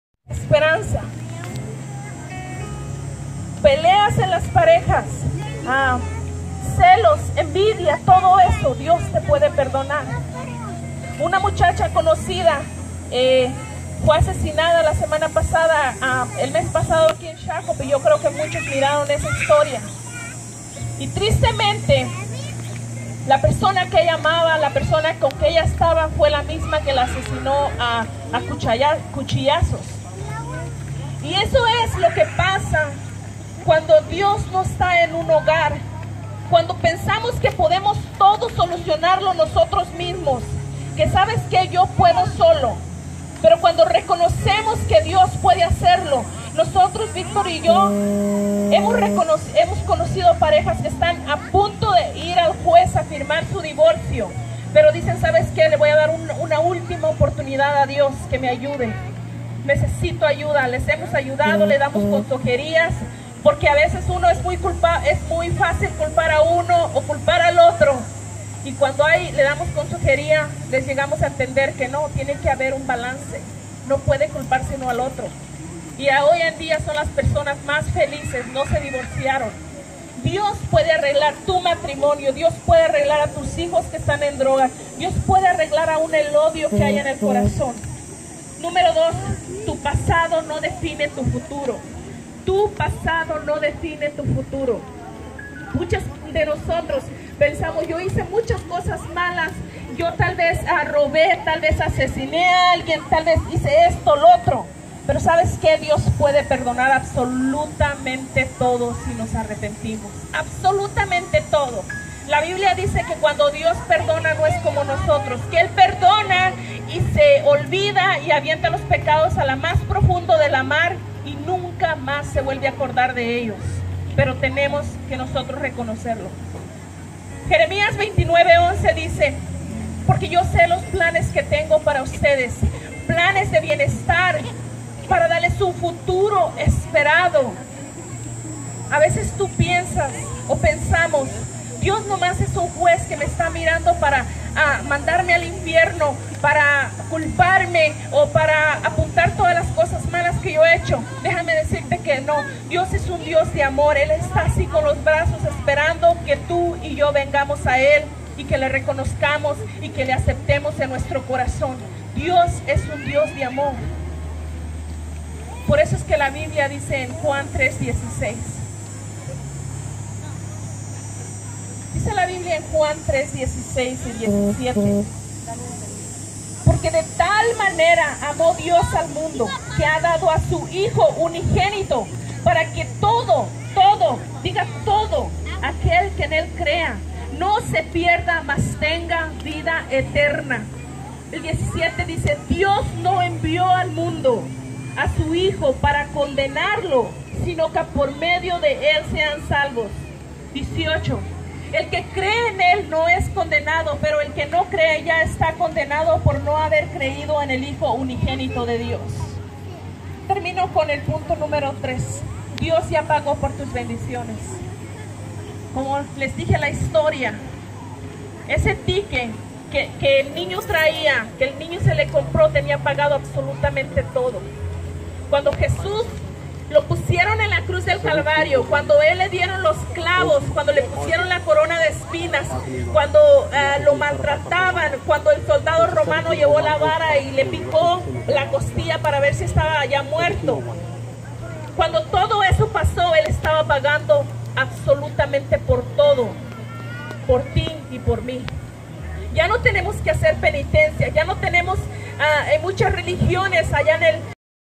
Pastora Daysi Martinez servicio aire libre
Regalamos más de 100 mochilas para niños en necesidad de North Minneapolis con la Pastora Daisy Martínez predicando la palabra